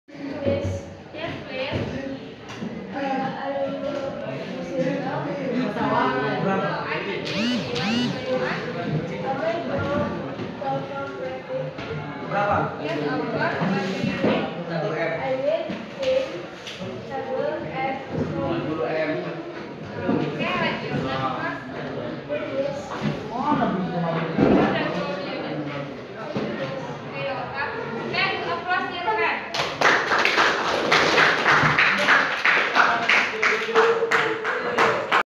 video pembelajaran English practice x Elka 1. it's about "how to borrow tools for practice"
S:Excuse me Miss
T: yes please,
S: are you busy right now?
T: no, Iam not busy. what's Going on?
S:may I borrow tools for practice?
T: yes, of course. what do you need?
S: i need tin, cable and screwdriver.
T: okay, write your name first.
S: done sir
T: here the tools do you need.
S: thank you sir
T: you're welcome